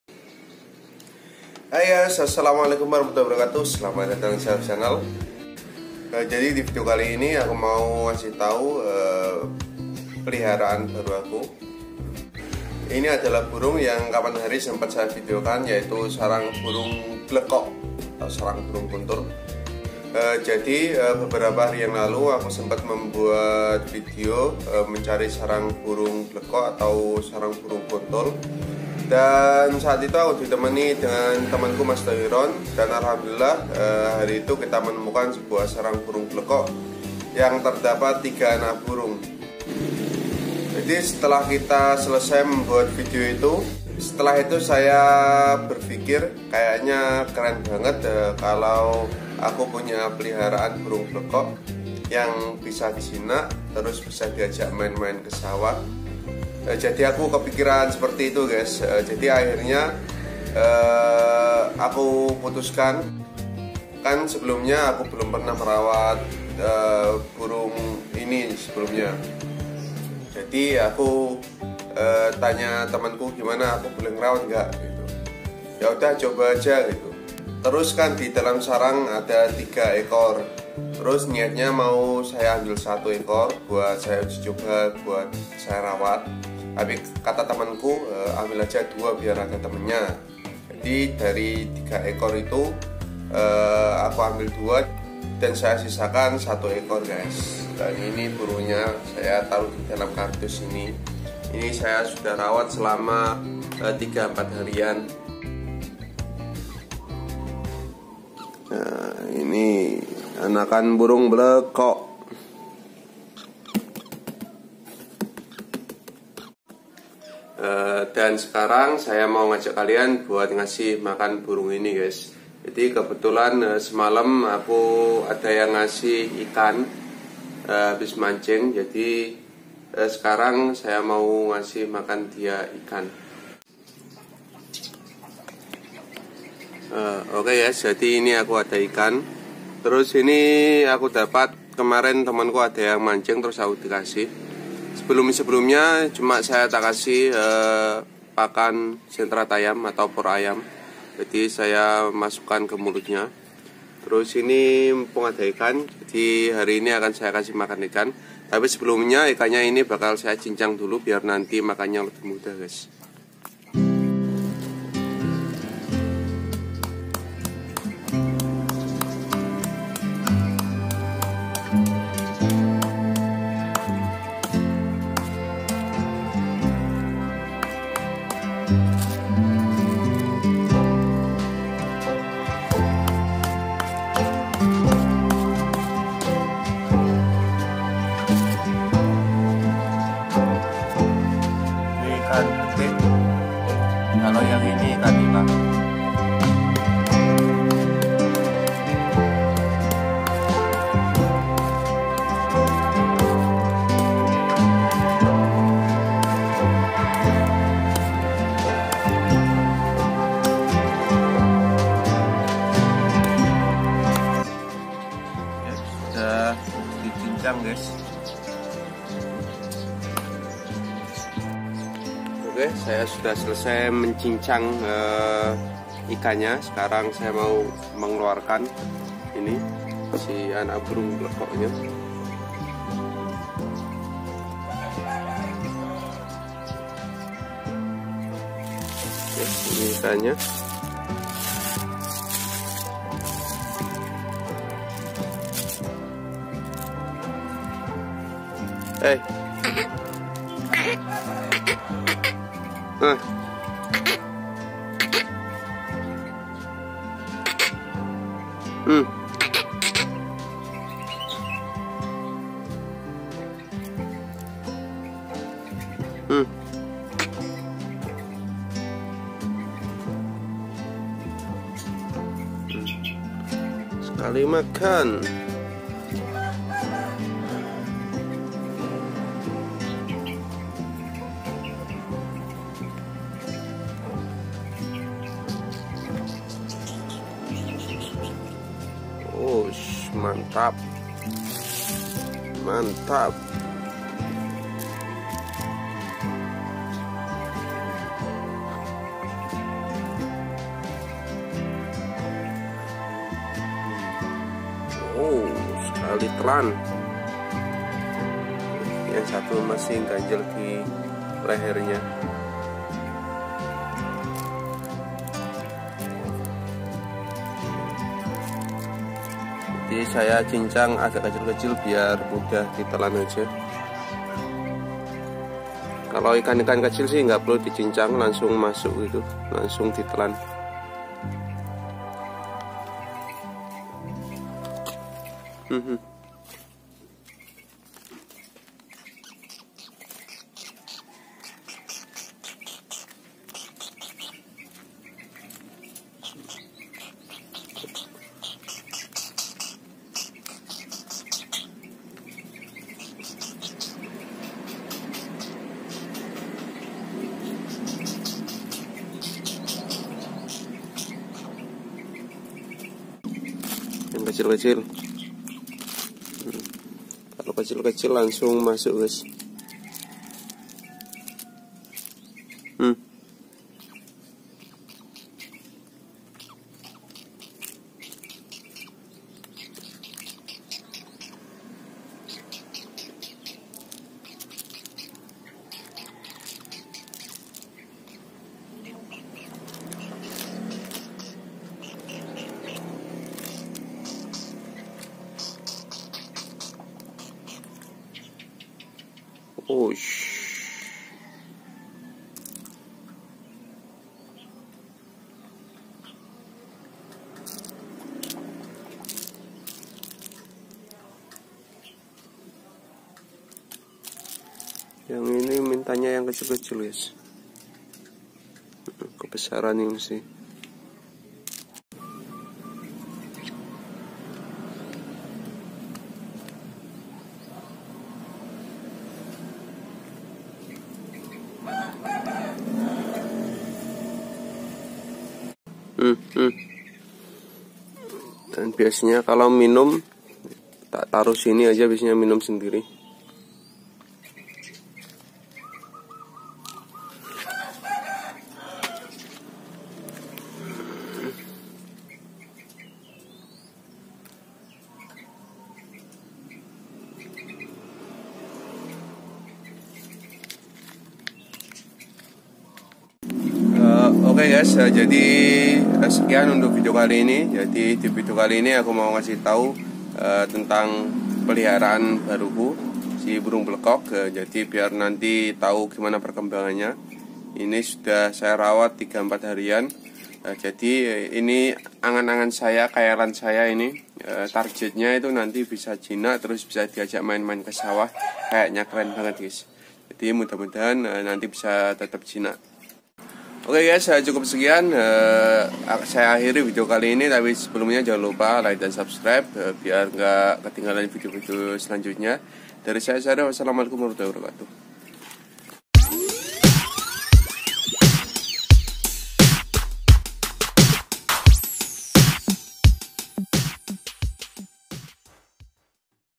Merawat Anakan Burung Blekok
merawat anakan burung blekok / kuntul. kapan hari saya sempat menemukan sarang burung blekok terdapat 3 anak burung. niatnya akan saya ambil satu buat saya rawat. seperti nya keren jika saya punya burung blekok jinak yang bisa di ajak main ke sawah. tapi akhirnya saya putuskan buat ambil 2 ekor biar ada temannya